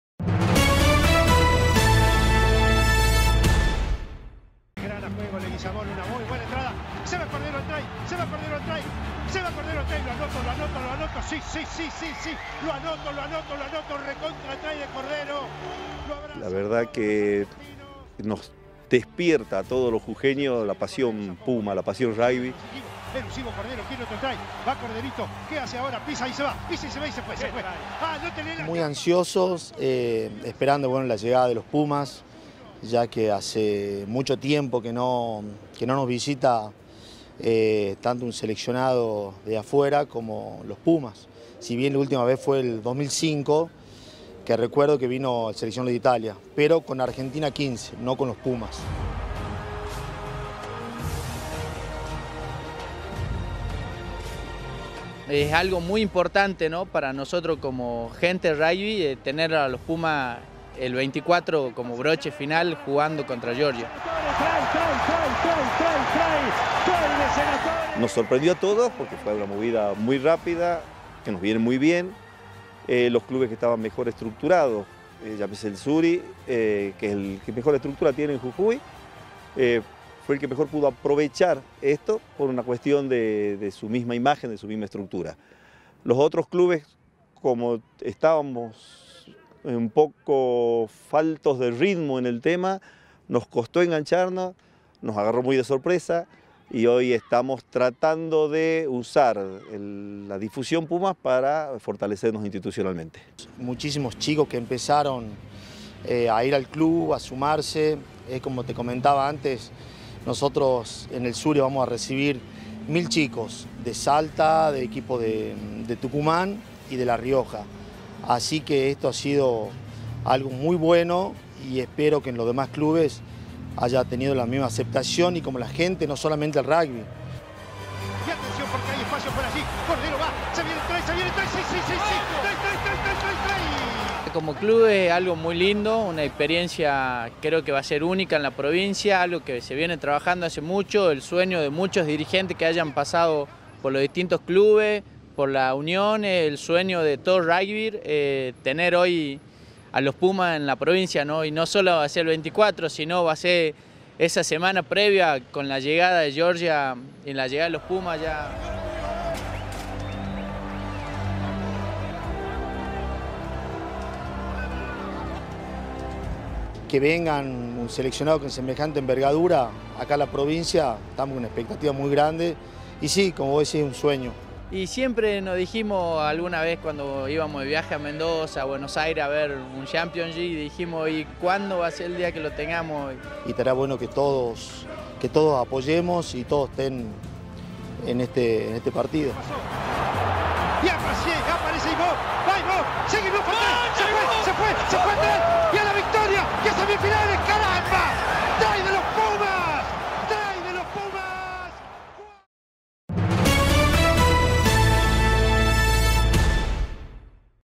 Llegan LosPumas y los jujeños se prenden
El 24 de junio el seleccionado argentino  juega en Jujuy frente a Georgia.